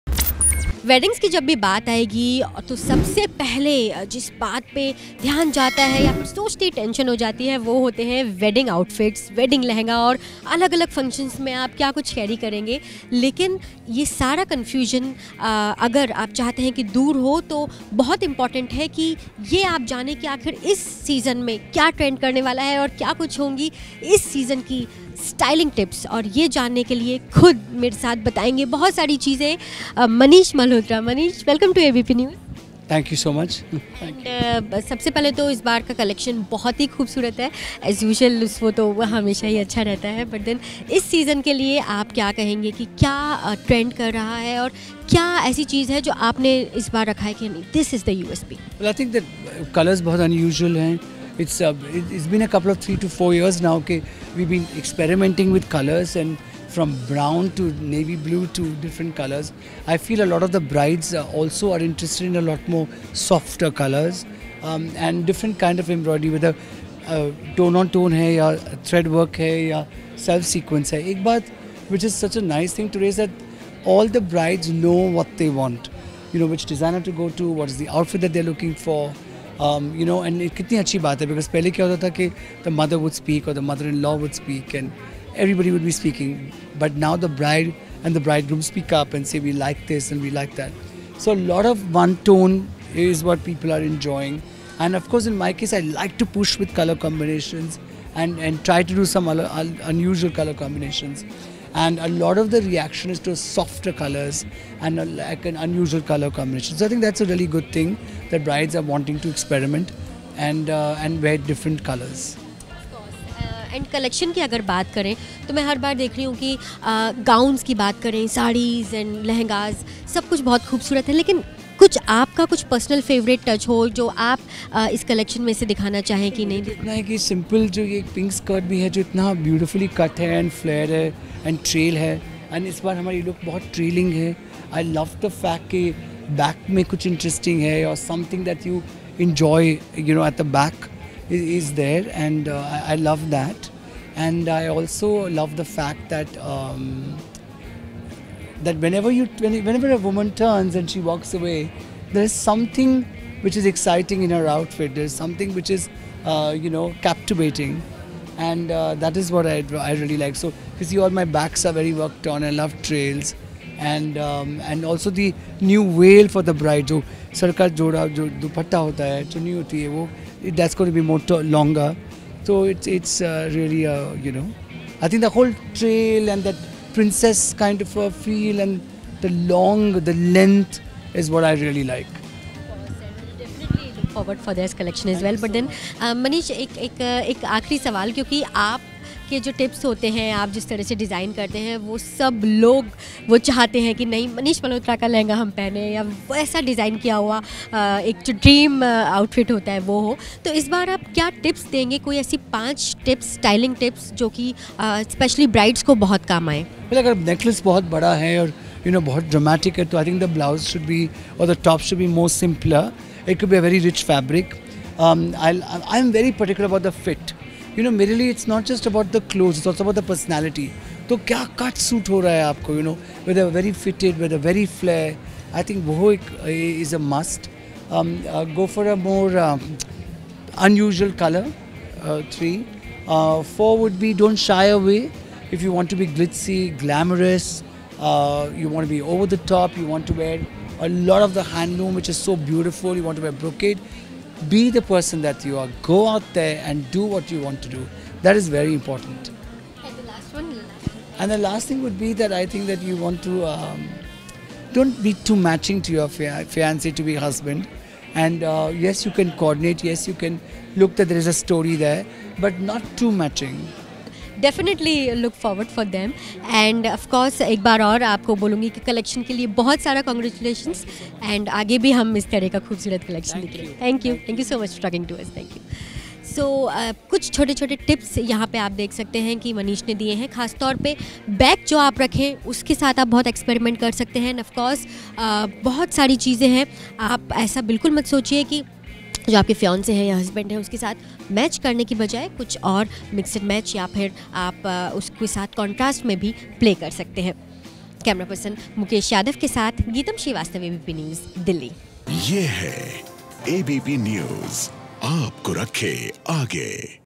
5 Tips For Brides From Manish Malhotra | ABP Uncut Beauty
Here are some trending designs for the would-be-brides by the ace Bollywood designer Manish Malhotra.